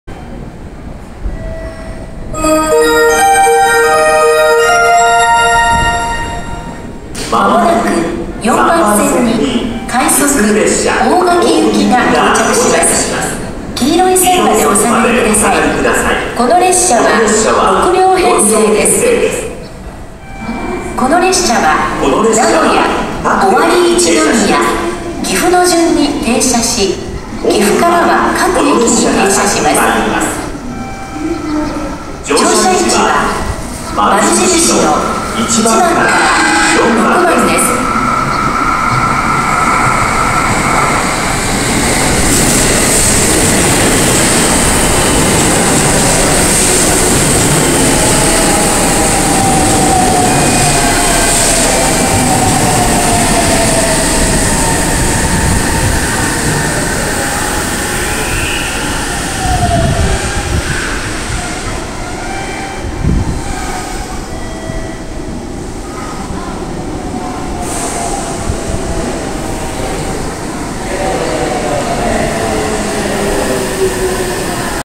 金山駅 快速 大垣行き 入線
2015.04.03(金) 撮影
クモハ―３１３系×６両編成の快速 大垣行きの入線を金山駅にて撮影しました。

※手持ちカメラで撮影致しました為、少々手振れが見受けれます。